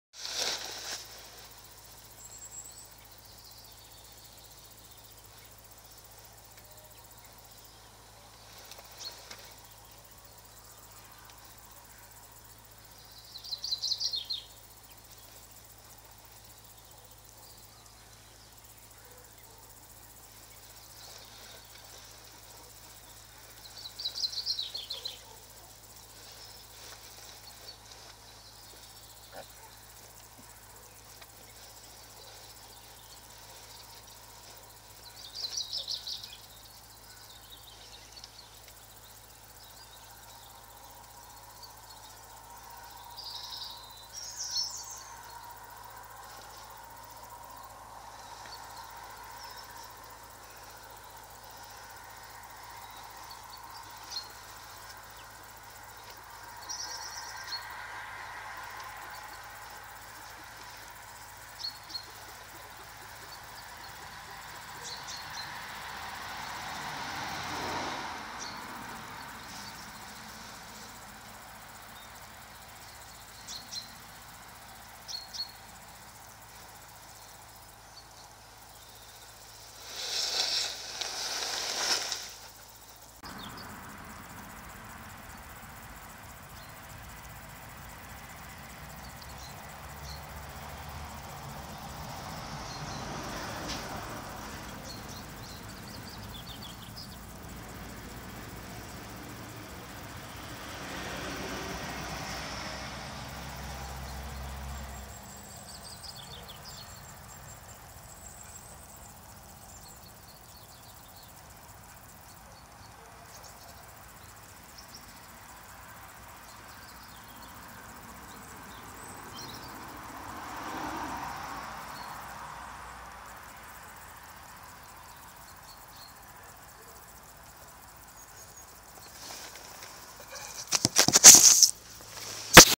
Lamb enjoys petting
Bottle-fed lamb allows me to stroke her and appears to enjoy it. She lifts her tail (I expect because her mother would have inspected it at feed time) and wags her tail (which she also does when being bottle-fed). Perhaps she associates this grooming with early maternal care. In the second shot, her ears go back as she extends her muzzle, but this does not appear to be a sign of a negative reaction (like when sheep are in pain); she also puts her ears back when feeding. My edit is simply to add both shots together to make one video.